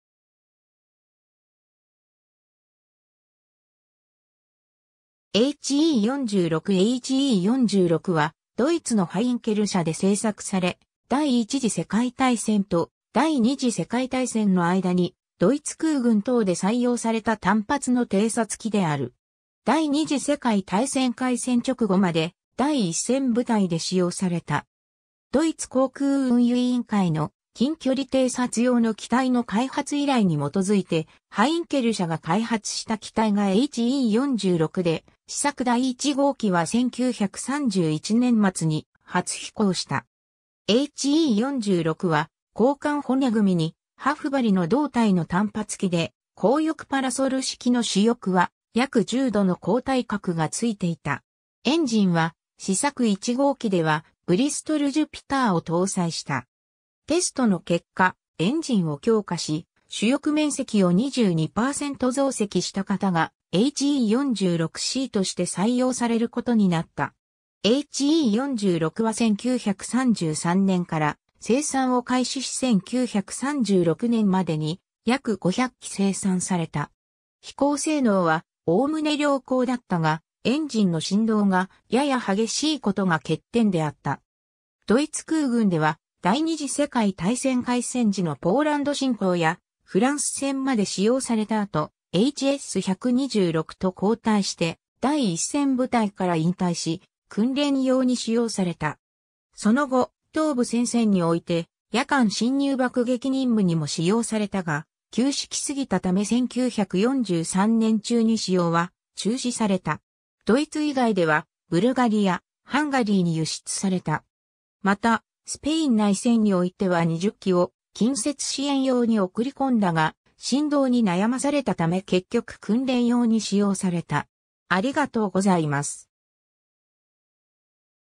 He 46 (航空機)
He 46 He 46 は、ドイツのハインケル社で製作され、第一次世界大戦と第二次世界大戦の間にドイツ空軍等で採用された単発の偵察機である。
第二次世界大戦開戦直後まで第一線部隊で使用された。
 ドイツ航空運輸委員会の近距離偵察用の機体の開発依頼に基づいてハインケル社が開発した機体がHe 46で、試作第1号機は1931年末に初飛行した。
He 46は、鋼管骨組に羽布張りの胴体の単発機で、高翼パラソル式の主翼は約10°の後退角がついていた。
エンジンは、試作1号機ではブリストル・ジュピターを搭載した。
テストの結果、エンジンを強化し主翼面積を22％増積した型がHe 46Cとして採用されることになった。
 He46は1933年から生産を開始し1936年までに約500機生産された。
飛行性能は概ね良好だったが、エンジンの振動がやや激しいことが欠点であった。
ドイツ空軍では第二次世界大戦開戦時のポーランド侵攻やフランス戦まで使用された後、Hs 126と交代して第一線部隊から引退し、訓練用に使用された。
その後、東部戦線において夜間侵入爆撃任務にも使用されたが、旧式過ぎたため1943年中に使用は中止された。
 ドイツ以外では、ブルガリア、ハンガリーに輸出された。
また、スペイン内戦においては20機を近接支援用に送り込んだが、振動に悩まされたため結局訓練用に使用された。